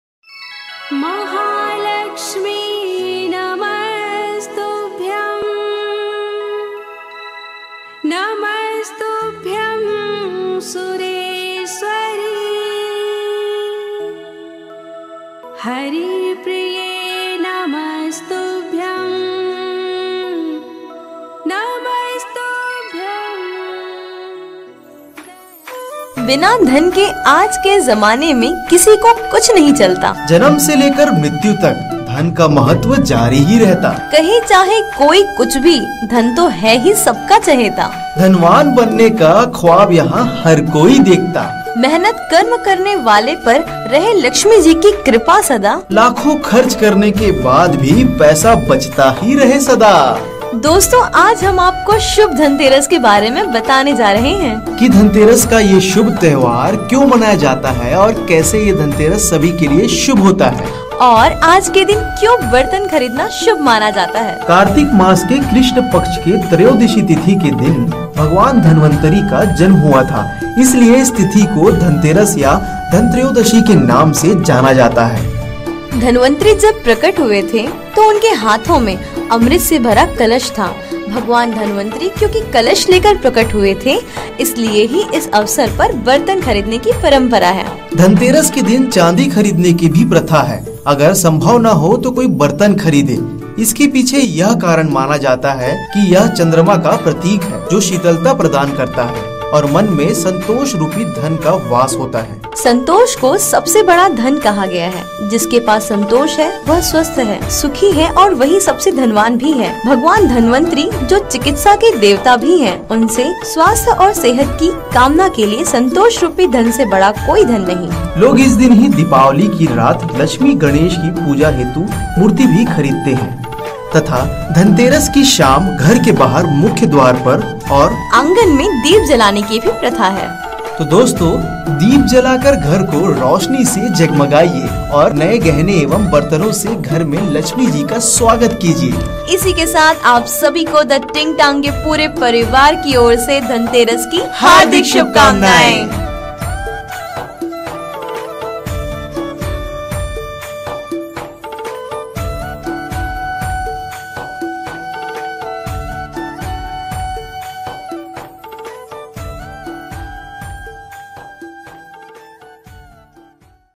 जानिए धनतेरस के दिन बर्तन एवं गहने खरीदना क्यों शुभ माना जाता हैं। सिर्फ the टिंग टांग न्यूज़ में ।
जानिए धनतेरस में क्या खरीदना चाहिए और किसकी पूजा करना चाहिए ।